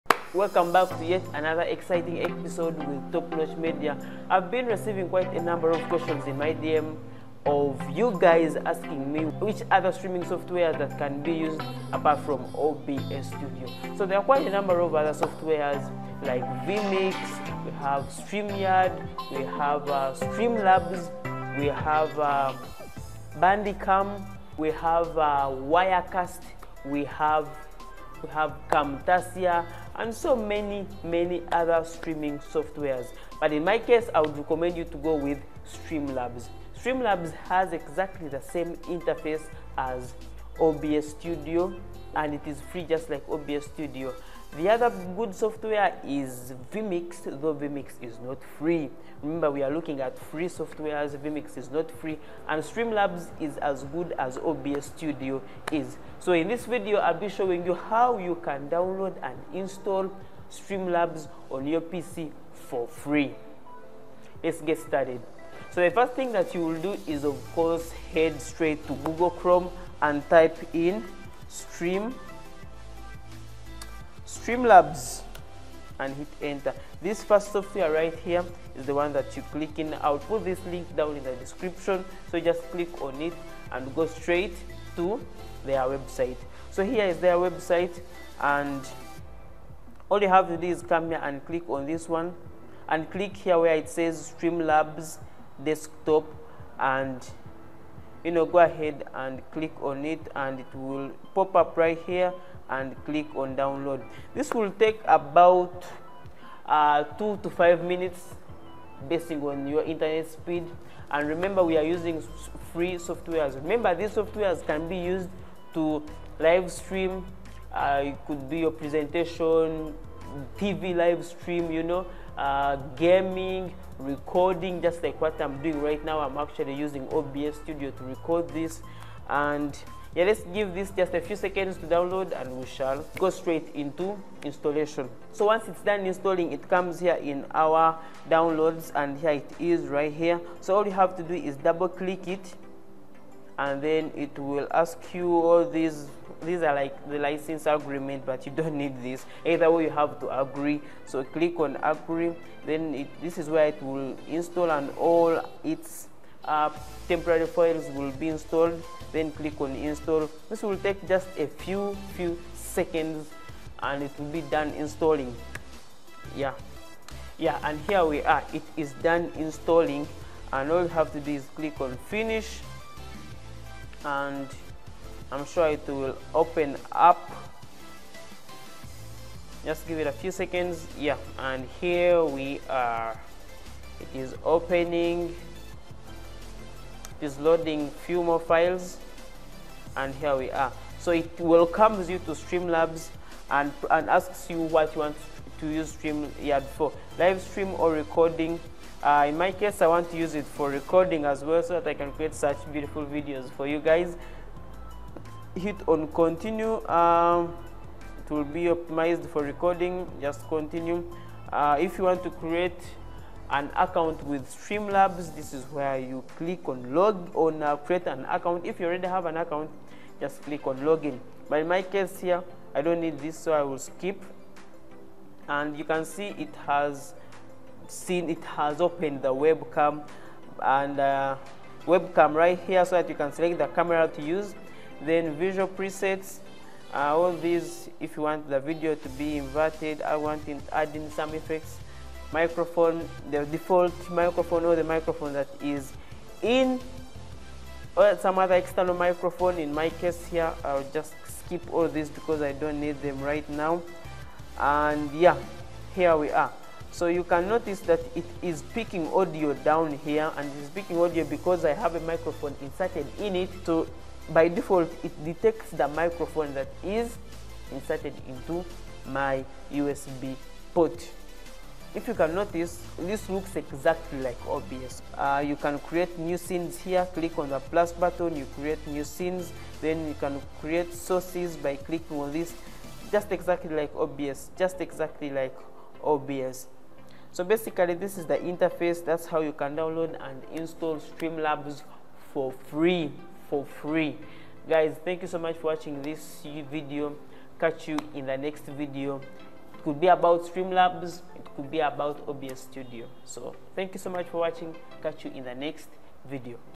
How to Download & Install Stream labs: The Perfect OBS Studio Alternative!
In this video, I'll walk you through a step-by-step guide on how to download and install Streamlabs, an easy-to-use alternative to OBS Studio for streaming and recording. Whether you're new to streaming or looking for a more user-friendly option, Streamlabs offers a streamlined experience with built-in features like themes, widgets, and more! Follow along and get started with Stream labs in just a few minutes.